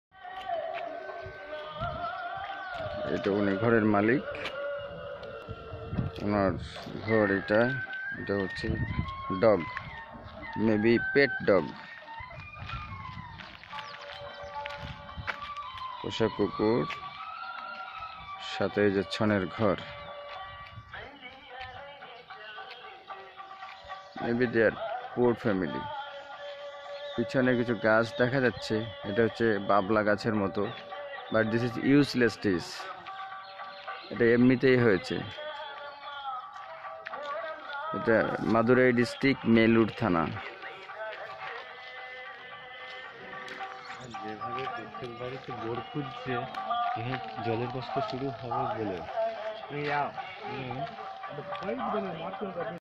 Traditional Tamil House 2 at Melur Madurai India
Traditional Tail house visited at Melur, Madurai, Tamil Nadu, India. Uncut Video dated 4 Aug 2019.